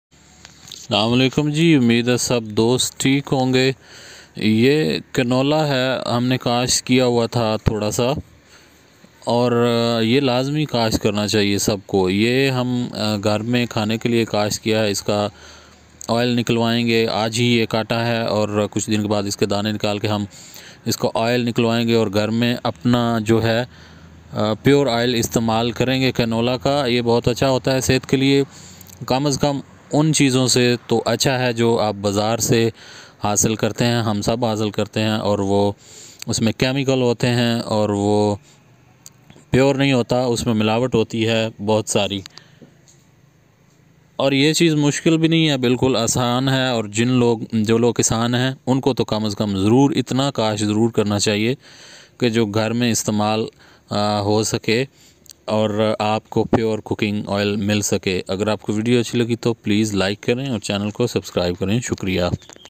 Canola Zaror Kasht Karna Chahiye || कैनोला की खेती करनी चाहिए || Naveed virk
In this video I talk about canola oil benefits and why to grow canola